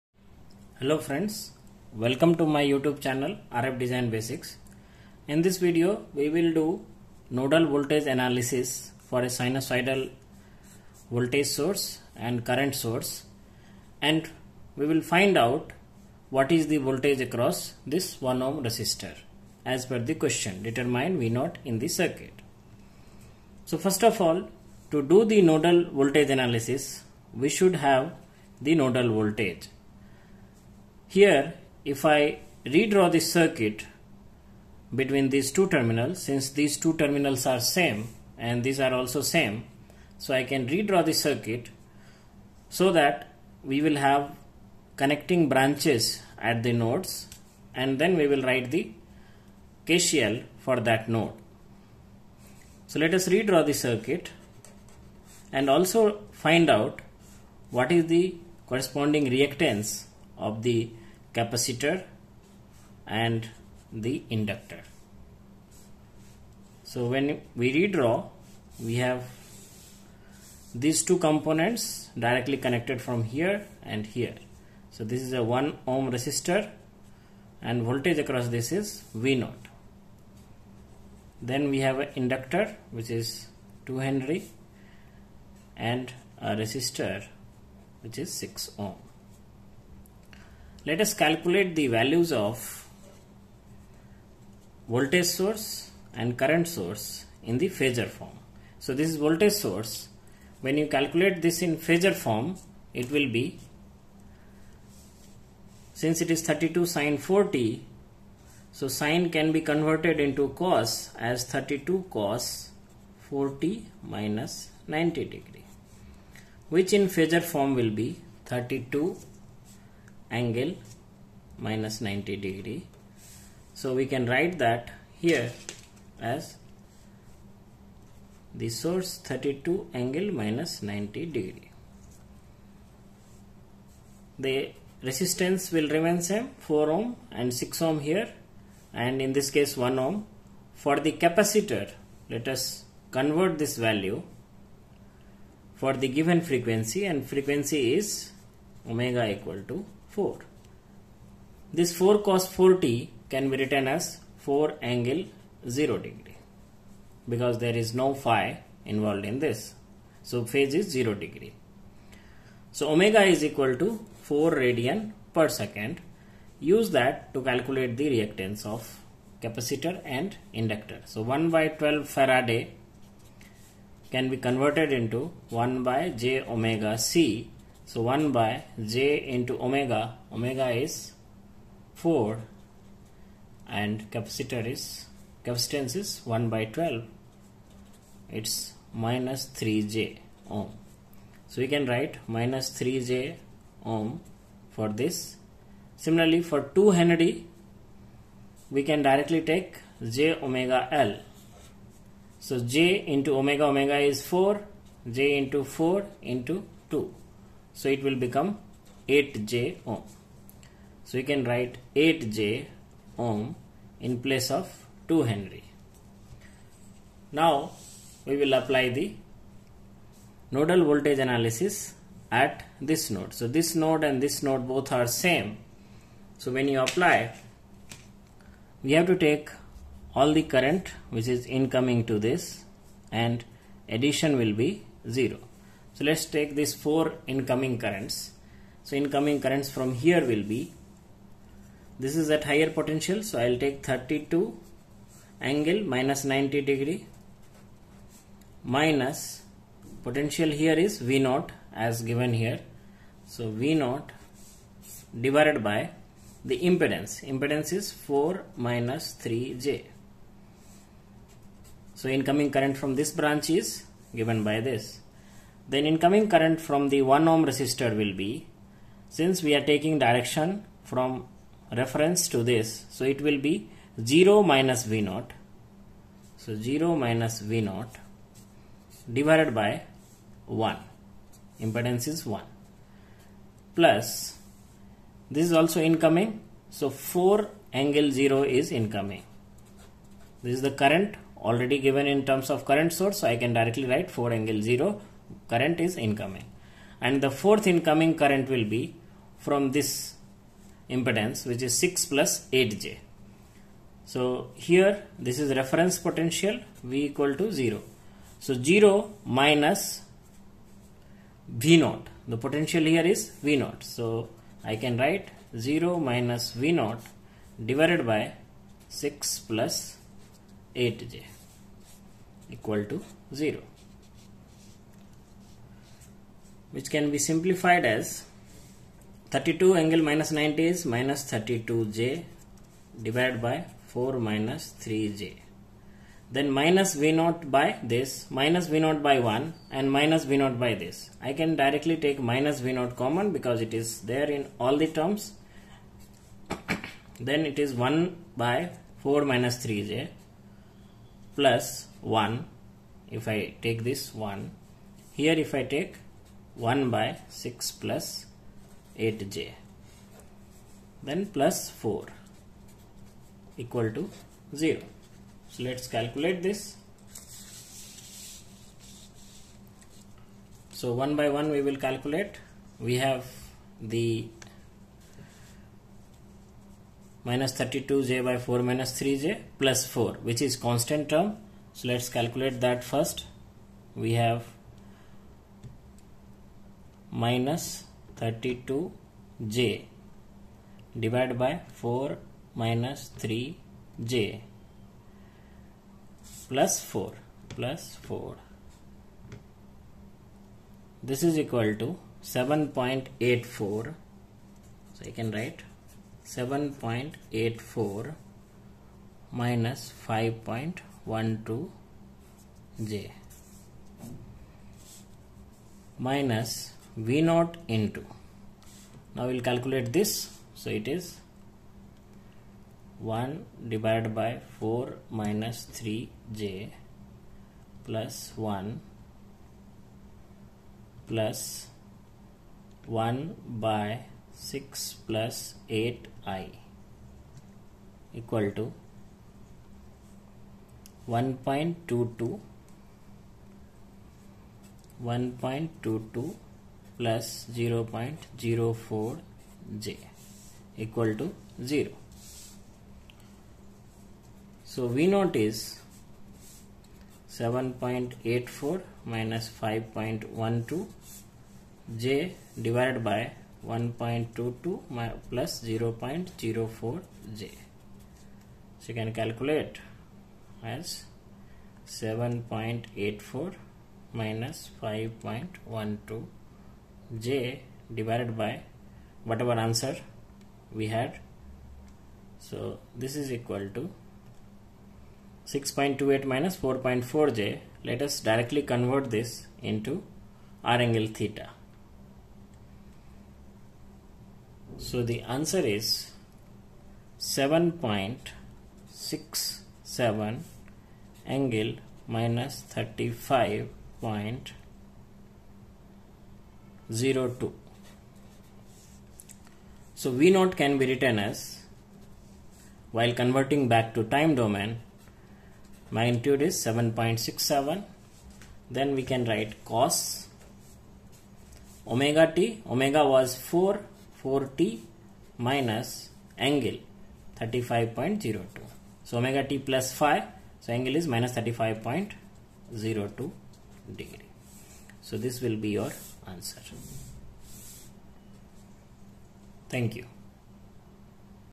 Problem solving using nodal voltage analysis for sinusoidal input | steady state analysis
In this video, nodal voltage analysis for sinusoidal steady state analysis is discussed with a problem.